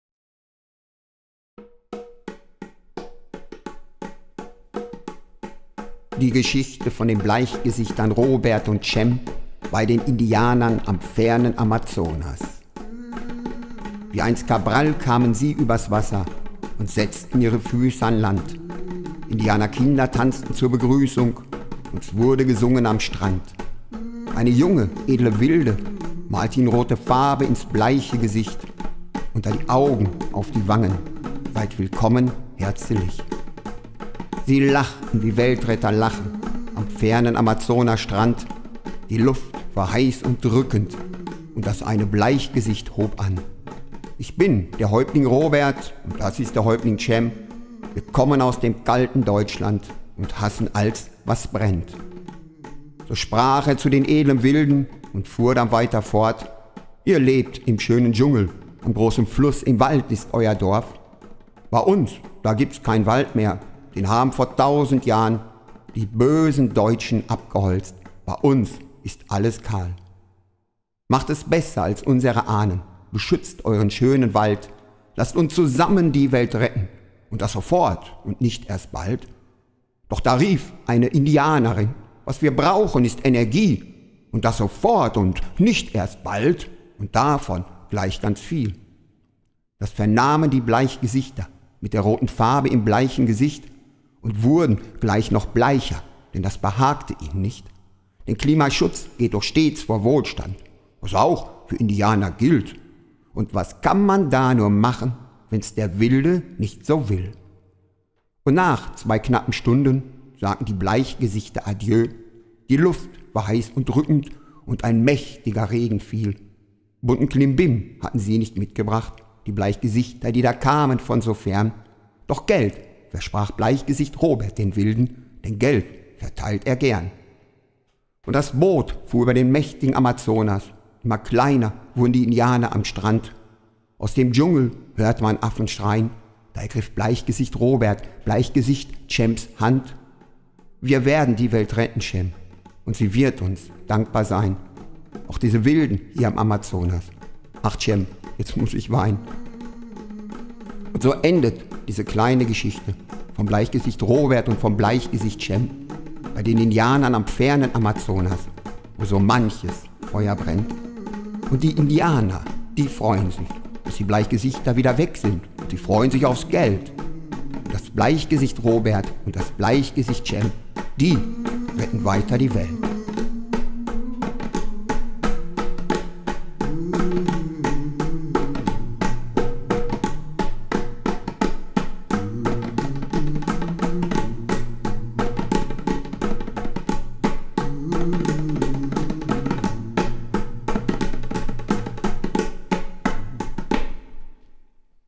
Die Geschichte von den Bleichgesichtern Robert und Cem bei den Indianern am fernen Amazonas
Wie einst Cabral kamen sie über 's Wasser
Und setzten ihre Füße an Land
Indianerkinder tanzten zur Begrüßung
Und 's wurde gesungen am Strand
Eine junge edle Wilde
Malte ihnen rote Farbe ins bleiche Gesicht
Unter die Augen, auf die Wangen
Seid willkommen herzelich

Sie lachten wie Weltretter lachen
Am fernen Amazonasstrand
Die Luft war heiß und drückend
Und das eine Bleichgesicht hob an
Ich bin der Häuptling Robert
Und das ist der Häuptling Cem
Wir kommen aus dem kalten Deutschland
Und hassen all's was brennt

So sprach er zu den edlen Wilden
Und fuhr dann weiter fort
Ihr lebt im schönen Dschungel
Am großen Fluss im Wald liegt euer Dorf
Bei uns da gibt 's keinen Wald mehr
Den haben vor 1000 Jahren
Die bösen Deutschen abgeholzt
Bei uns ist alles kahl

Macht es besser als unsere Ahnen
Beschützt euren schönen Wald
Lasst uns zusammen die Welt retten
Und das sofort und nicht erst bald ...
Doch da rief eine Indianerin
Was wir brauchen ist Energie
Und davon gleich ganz viel

Das vernahmen die Bleichgesichter
Mit der roten Farbe im bleichen Gesicht
Und wurden gleich noch bleicher
Denn das behagte ihnen nicht
Denn Klimaschutz geht doch stets vor Wohlstand
Was auch für Indianer gilt
Und was kann man da nur machen
Wenn 's der Wilde nicht so will

Und nach zwei knappen Stunden
Sagten die Bleichgesichter den Indianern adieu
Und ein mächtiger Regen fiel
Bunten Klimbin hatten sie nicht mitgebracht
Die Bleichgesichter die da kamen von so fern
Doch Geld versprach Bleichgesicht Robert den Wilden
Denn Geld verteilt er gern

Und das Boot fuhr über den mächtigen Amazonas
Immer kleiner wurden die Indianer am Strand
Aus dem Dschungel hörte man Affen schreien
Da ergriff Bleichgesicht Robert Bleichgesicht Cem 's Hand
Wir werden die Welt retten, Cem
Und sie wird uns dankbar sein
Auch diese Wilden hier am Amazonas
Ach Cem, jetzt muss ich weinen

Und so endet diese kleine Geschichte
Von Bleichgesicht Robert und von Bleichgesicht Cem
Bei den Indianern am fernen Amazonas
Wo so manches Feuer brennt
Und die Indianer die freuen sich
Dass die Bleichgesichter wieder weg sind und sie freuen sich auf 's Geld
Und das Bleichgesicht Robert und das Bleichgesicht Cem
Die retten weiter die Welt